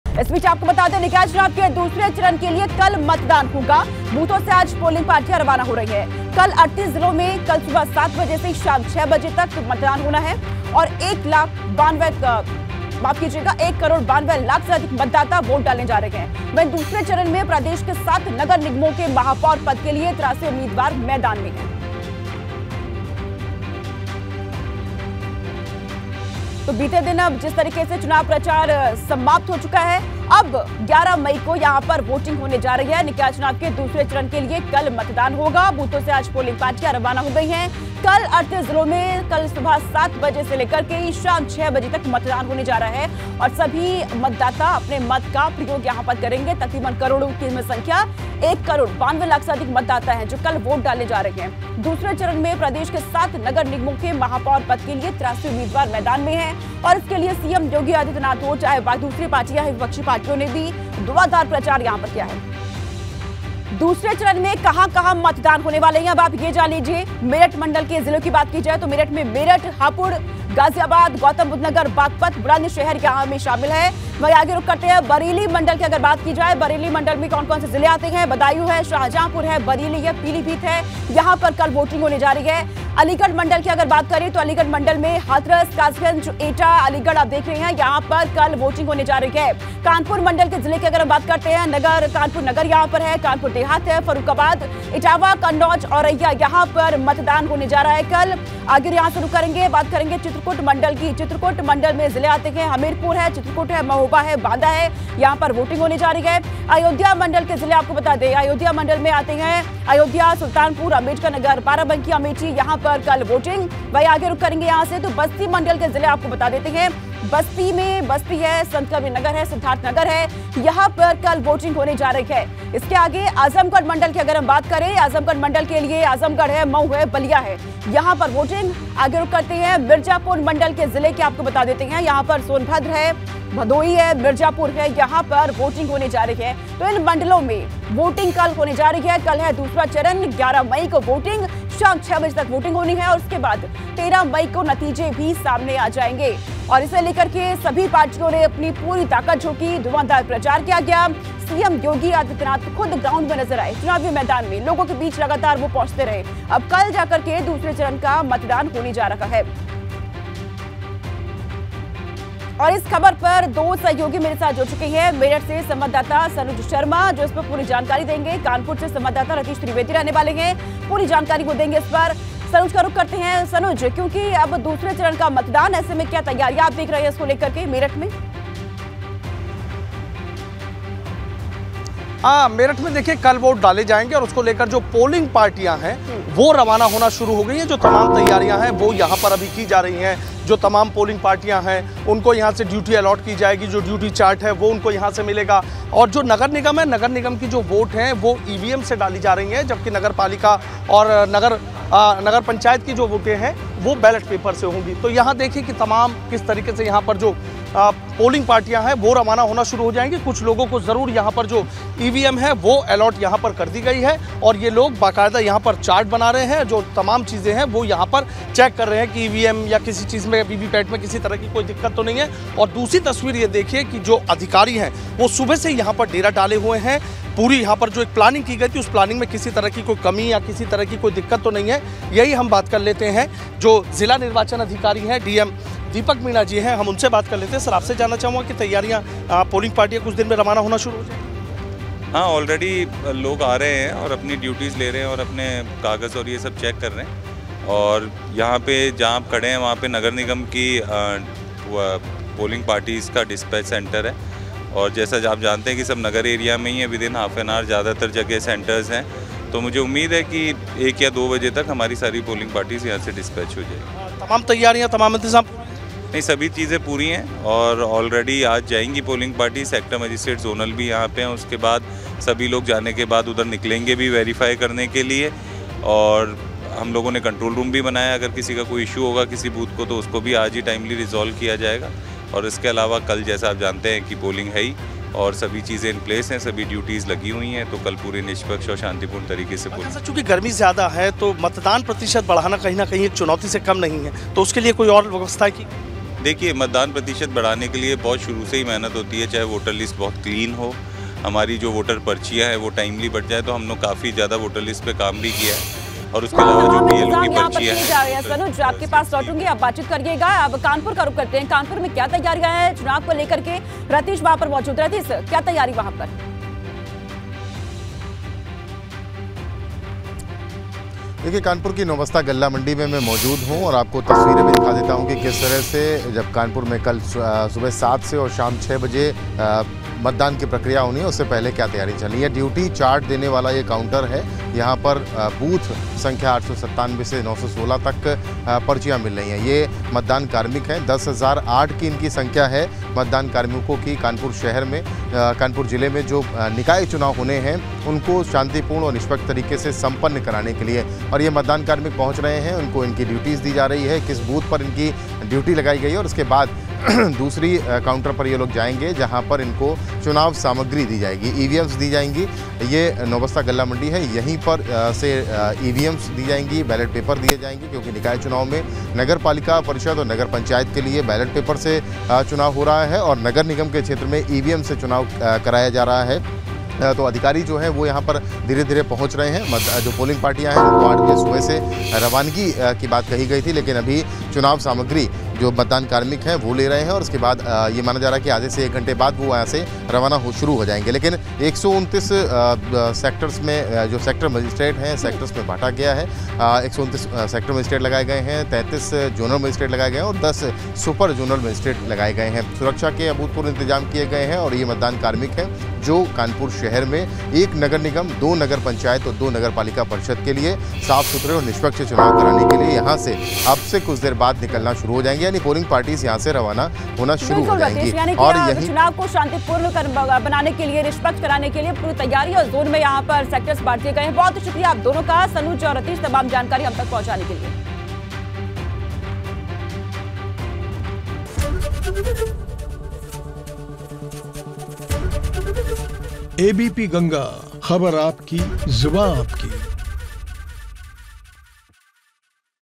UP Nikay Chunav : निकाय चुनाव के दूसरे चरण का मतदान कल, पोलिंग पार्टियां आज बूथों के लिए होगी रवाना
निकाय चुनाव के दूसरे चरण के लिए कल मतदान होना है ऐसे में सभी पोलिंग पार्टियां आज रवाना हो जाएगी.